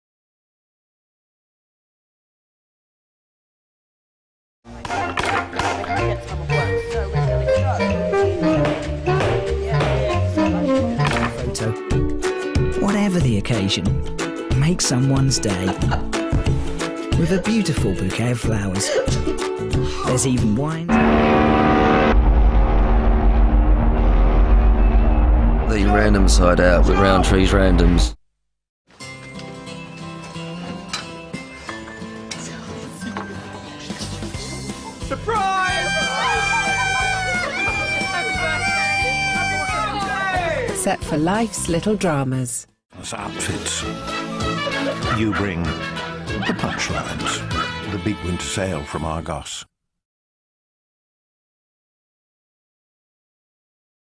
Rosa Marie Lewis - Commercial Showreel -
Our Client : "Rosa Marie Lewis" - Commercial Showreel

A collection of adverts our client Rosa has featured in.
Terry's Chocolate Orange, Moonpig, Rowntree's Randoms, Silvikrin Argos